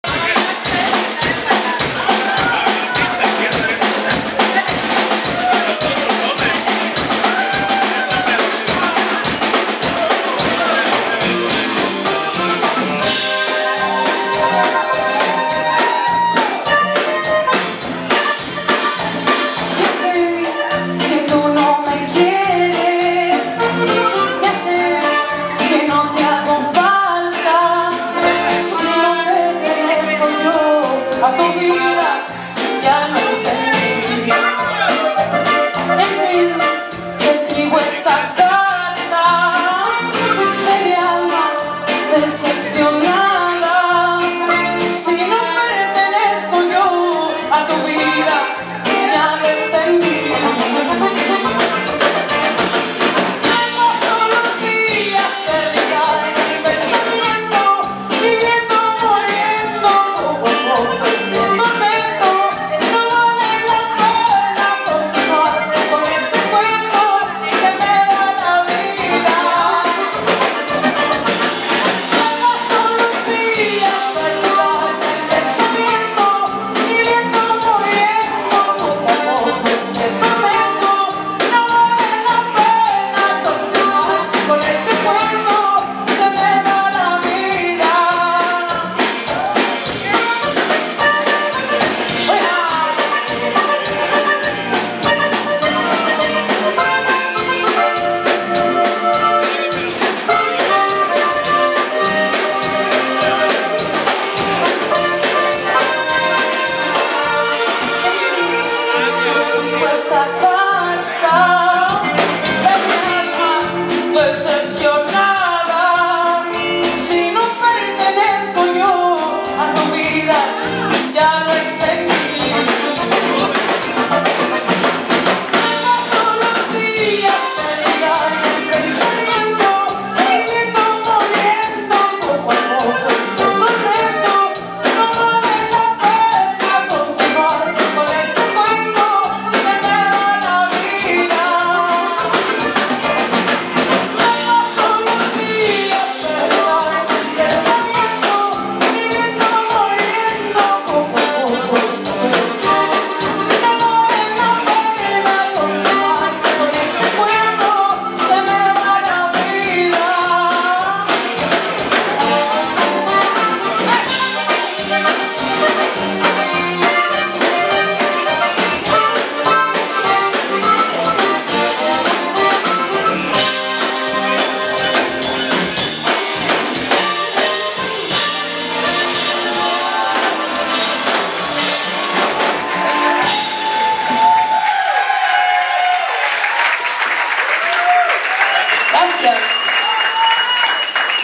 La Santa Cecilia at The Kessler Theater in Dallas, Texas
Recorded live in June of 2014 at the historic Kessler Theater in X+/North Oak Cliff